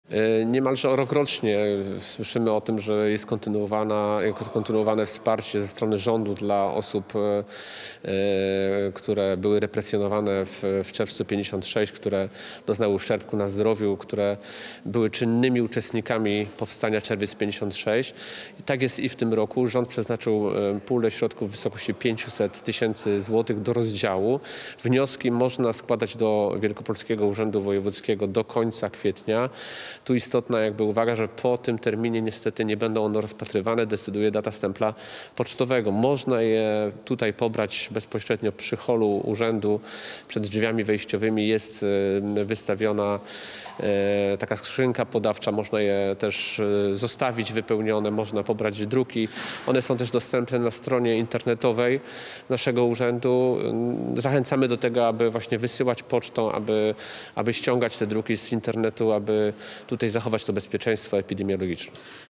Pomoc dla kombatantów
Rząd przeznaczy pół miliona złotych dla kombatantów wydarzeń poznańskiego czerwca 56 roku. Pieniądze trafią do osób, które w wyniku wydarzeń sprzed 65 lat - doznały uszczerbku na zdrowiu, represji ze strony funkcjonariuszy aparatu bezpieczeństwa, spotkały się z represjami w miejscu pracy, bądź miały szczególne zasługi w niesieniu pomocy ofiarom poznańskiego buntu. O szczegółach akcji mówi rzecznik wojewody - Tomasz Stube.